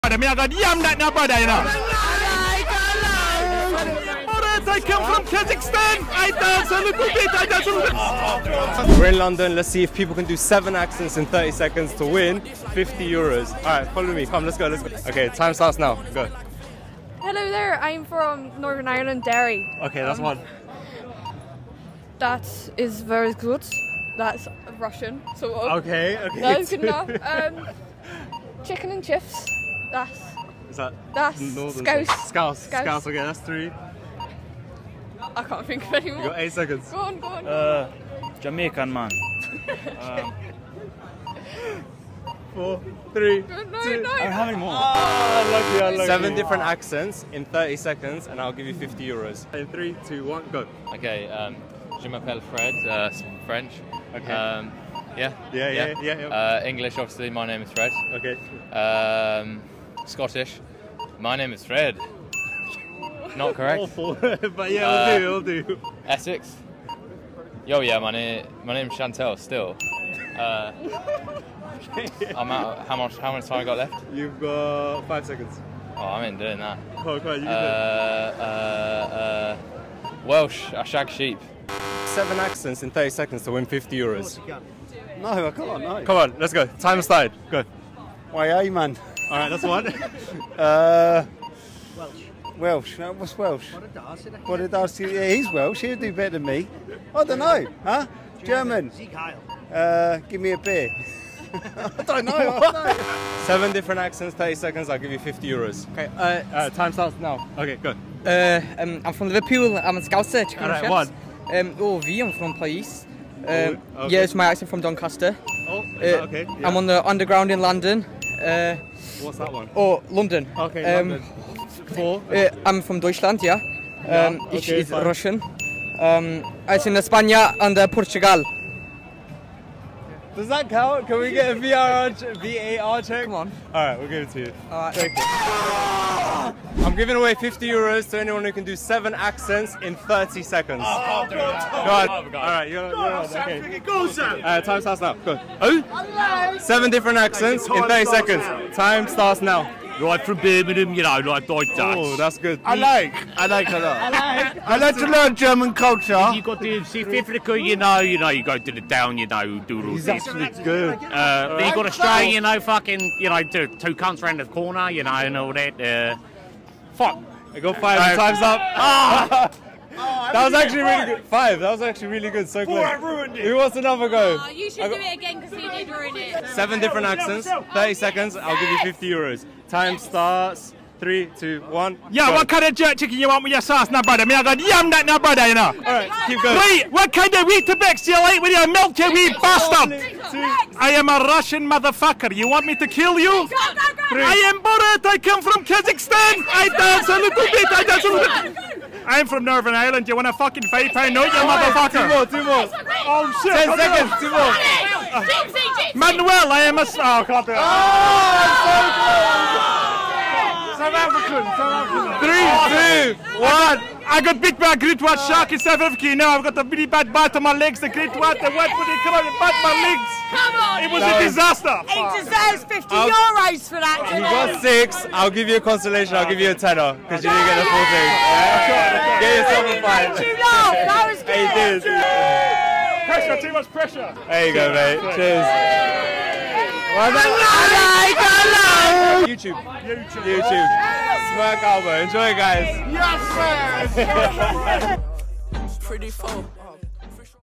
How Many Accents Can You Do in 30 Seconds?
We Challenged British People to do 7 Different Accents in 30 Seconds to Win €50! This is how it went 🤣